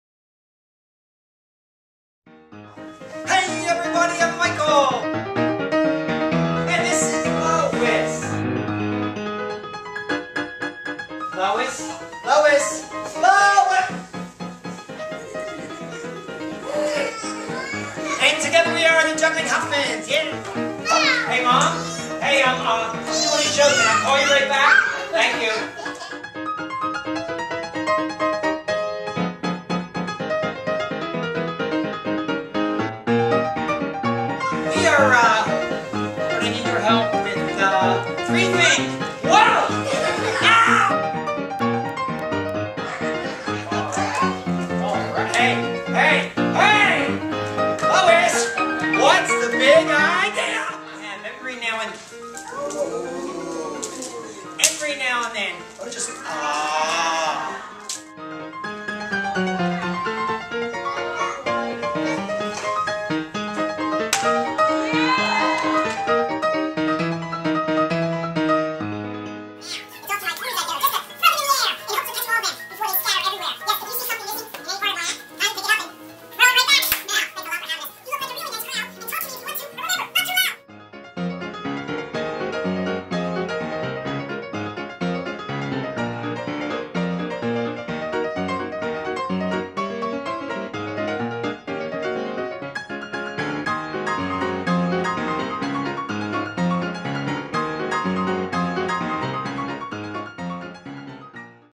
The Juggling Hoffmans
We're the Juggling Hoffmans, and this is a new demo for our show, perfect for kids and adults! We perform at birthday parties, libraries, corporate events, or any celebration you're throwing!

Created by: Nick Hoffman